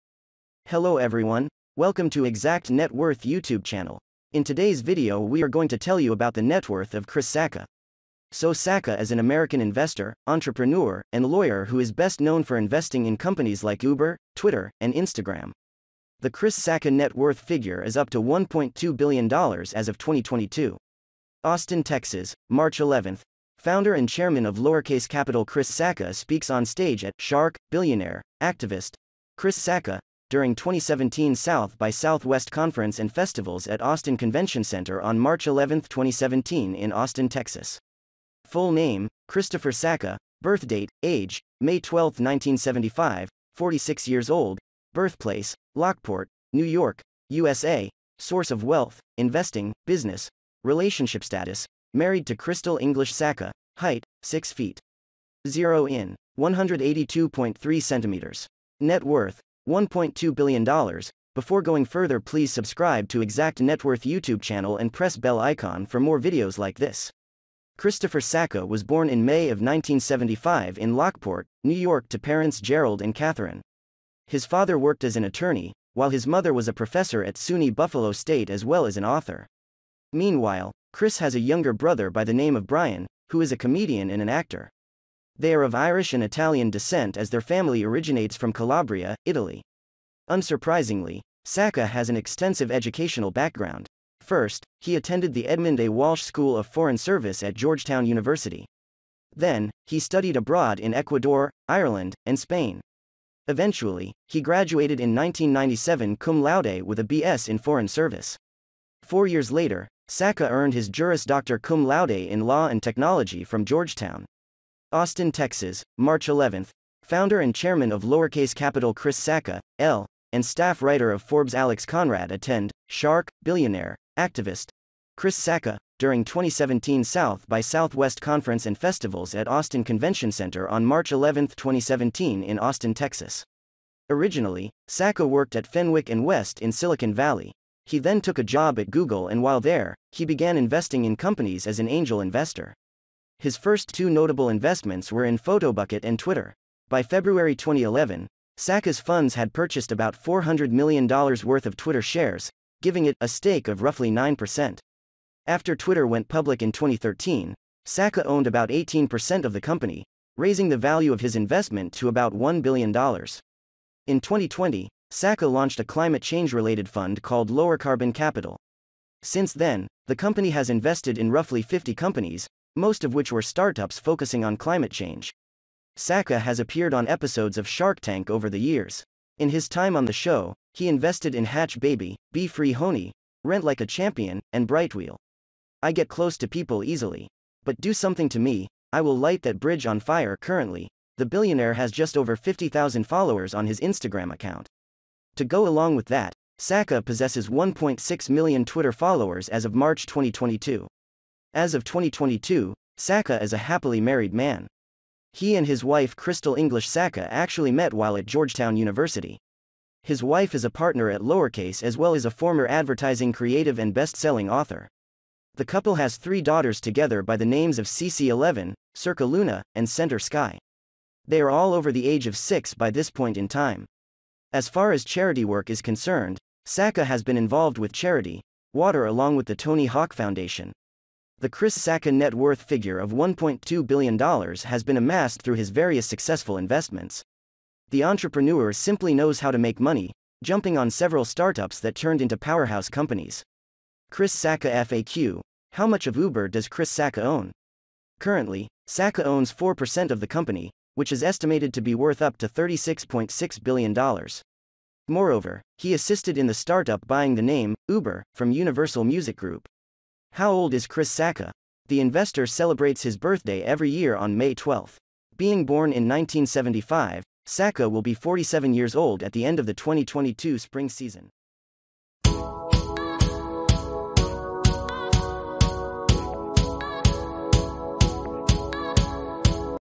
Chris Sacca Net Worth - Chris Sacca Interview - Chris Sacca Shark Tank - Chris Sacca Story
Sacca is an American investor, entrepreneur, and lawyer who is best known for investing in companies like Uber, Twitter, and Instagram. The Chris Sacca net worth figure is up to $1.2 billion as of 2022.